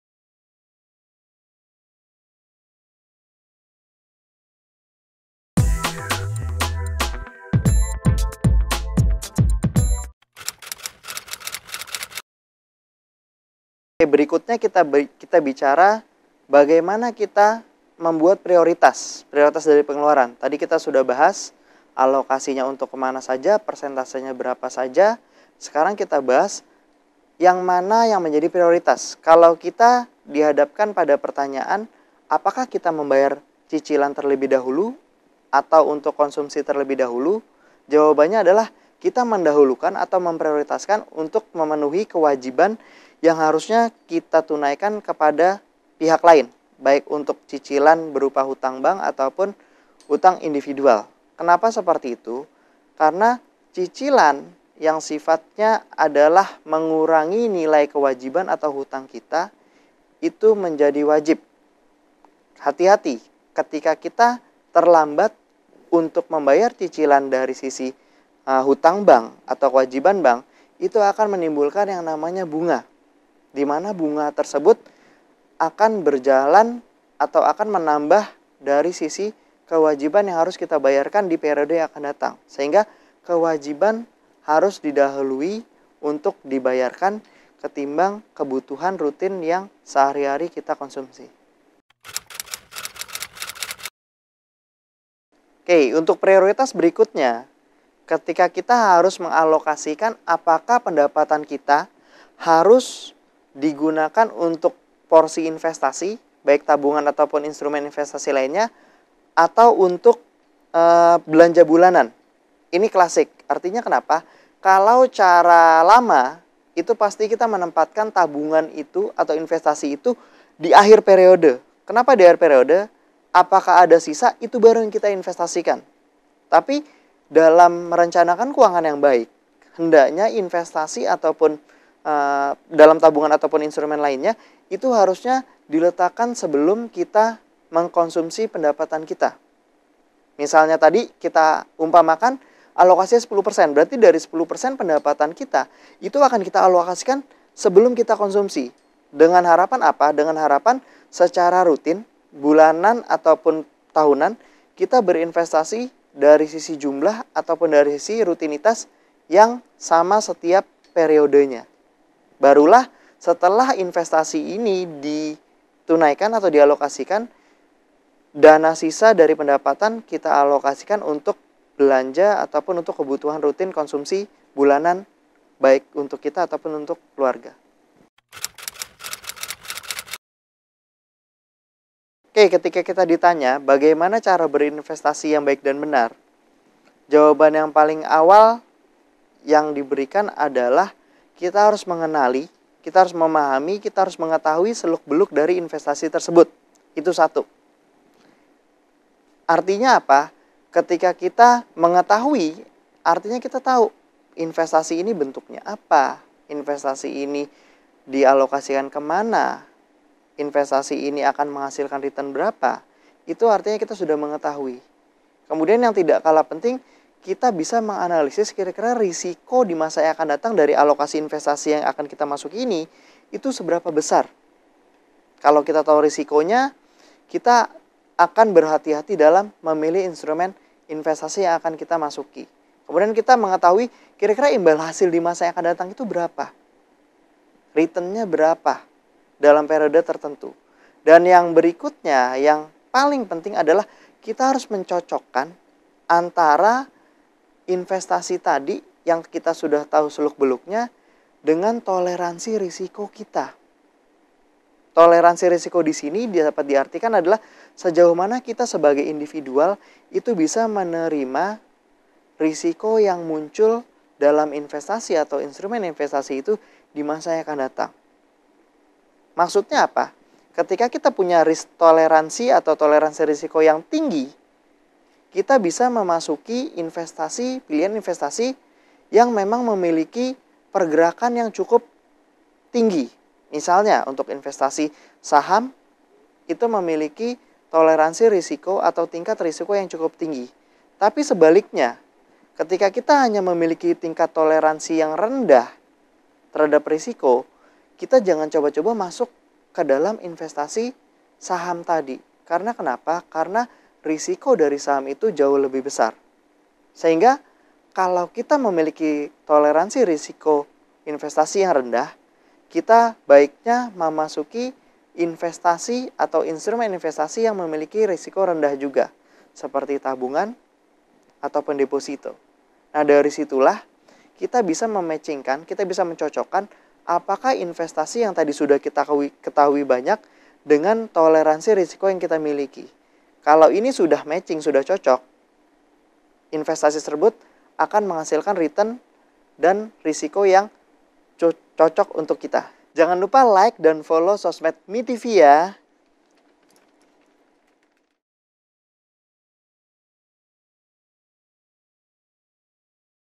FAQ Eps. 11 - Pengaturan Uang part 2
Sudah sampai di FAQ Pengaturan Keuangan Part 2 nih, sobat MITV! Di video kali ini, ada 3 poin yang akan dibahas. Poin pertama, lebih baik membayar cicilan dulu atau belanja kebutuhan bulanan? Kedua, lebih baik menabung+investasi dulu atau belanja kebutuhan bulanan ya? Ketiga, bagaimana sih cara investasi yang baik? YUK MAKANYA CARI TAU JAWABANNYA DI SINI! :D